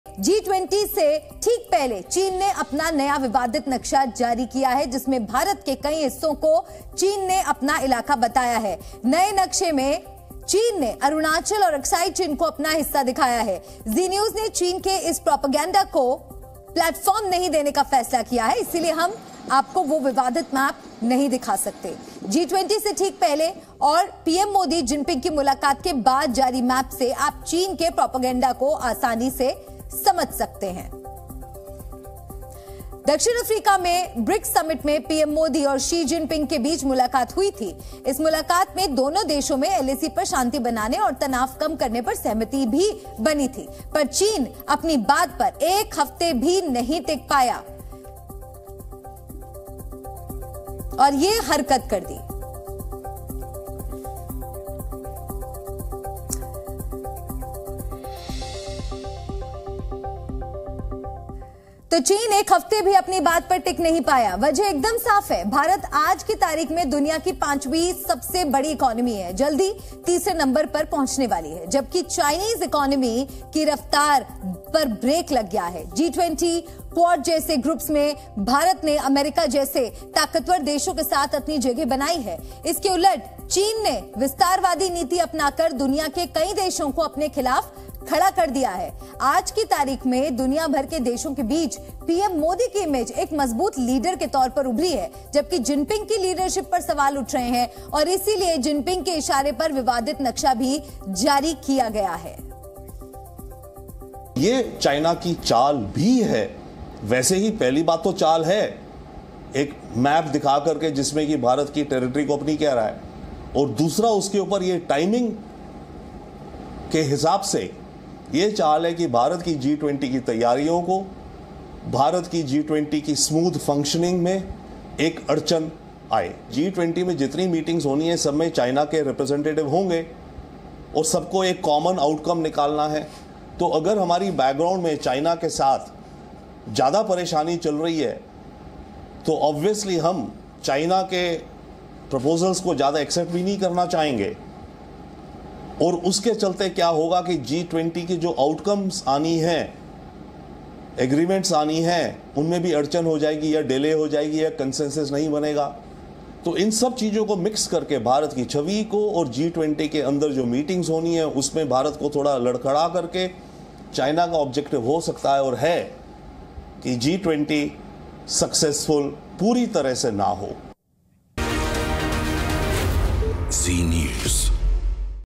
China Big Mistake Before G20: G20 से पहले चीन की गुस्ताखी | Breaking | India China Border Conflict
चीन ने आधिकारिक तौर पर अपने "मानक मानचित्र" का 2023 संस्करण जारी किया है, जो अरुणाचल प्रदेश राज्य और अक्साई चिन क्षेत्र को उसके क्षेत्र के हिस्से के रूप में दिखाता है।
28 अगस्त को जारी मानचित्र में अरुणाचल प्रदेश को दिखाया गया है, जिस पर चीन दक्षिण तिब्बत होने का दावा करता है और अक्साई चिन पर 1962 के युद्ध में उसने कब्जा कर लिया था। नए नक्शे में ताइवान और विवादित दक्षिण चीन सागर को भी चीनी क्षेत्र में शामिल किया गया है।

China has officially released the 2023 edition of its “standard map,” which shows the state of Arunachal Pradesh and the Aksai Chin region as part of its territory.
The map released on August 28 shows Arunachal Pradesh which China claims as South Tibet and Aksai Chin occupied by it in the 1962 war as part of its territory. Taiwan and the disputed South China Sea are also included within the Chinese territory in the new map.